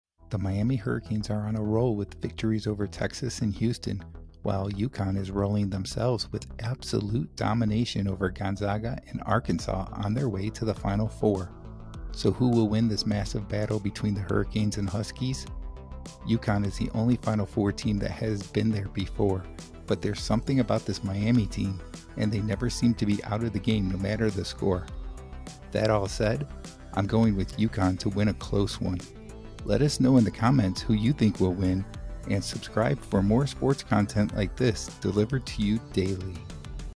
Miami UConn Final 4 Preview 🔥🏀
A preview of the Final Four matchup between the Miami Hurricanes and the UCONN Huskies.